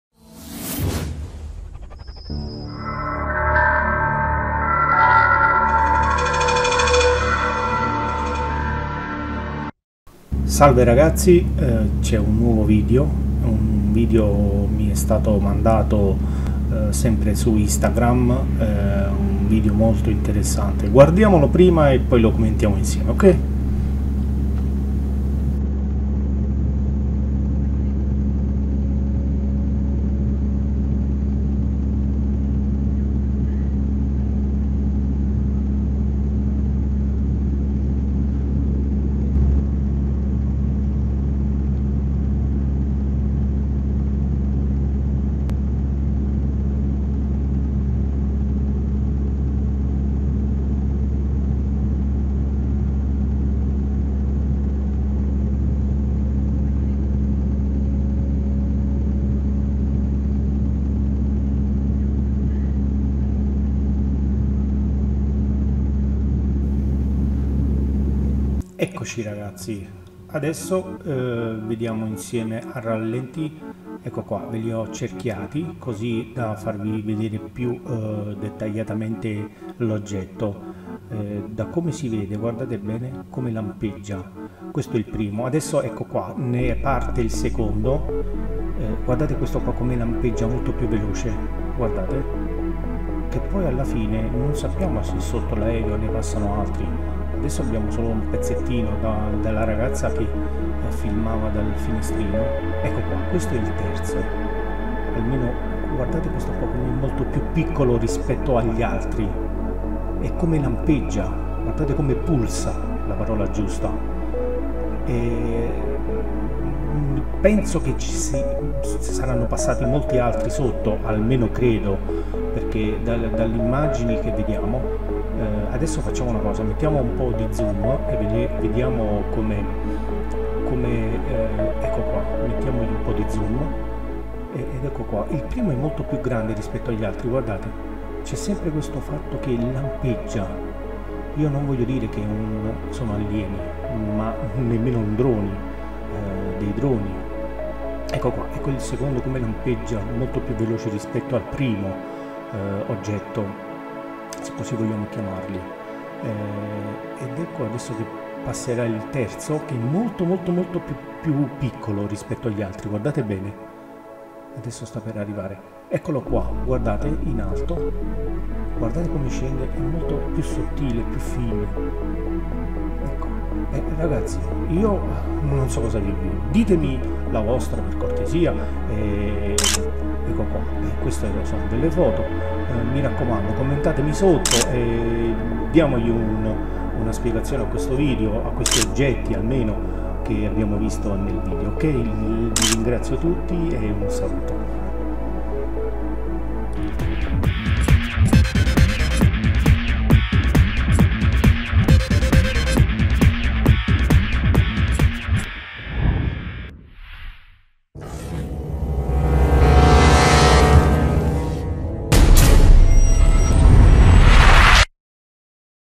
Una ragazzina filma una flottiglia di Ufo! 22/01/2021
Una ragazzina in gita con la famiglia filma una una flottiglia di oggetti volanti non identificati, a parer mio sono sonde aliene.
Il video mi è stato mandato su instagram dal papà della ragazza, voleva un mio parere sul filmato, dalle analisi del filmato deduco che si trattano di sonde aliene, ho caricato questo video per condividerlo con voi, perchè il vostro parere mi interessa.

ENGLISH
A girl on a trip with her family films a flotilla of unidentified flying objects, in my opinion they are alien probes.
The video was sent to me on instagram by the girl's father, he wanted my opinion on the video, from the analysis of the video I deduce that they are alien probes, I uploaded this video to share it with you, because your opinion interests me.

SPAGNOLO
Una chica en un viaje con su familia filma una flotilla de objetos voladores no identificados, en mi opinión son sondas extraterrestres.
El video me lo envió en instagram el padre de la niña, quería mi opinión sobre el video, del análisis del video deduzco que son sondas alienígenas, subí este video para compartirlo contigo, porque tu opinión me interesa .

RUSSO
Девушка в поездке с семьей снимает флотилию неопознанных летающих объектов, на мой взгляд, это инопланетные зонды.
Видео прислал мне в инстаграм отец девушки, он хотел узнать мое мнение о видео, из анализа видео я сделал вывод, что это инопланетные зонды, я загрузил это видео, чтобы поделиться им с вами, потому что ваше мнение меня интересует .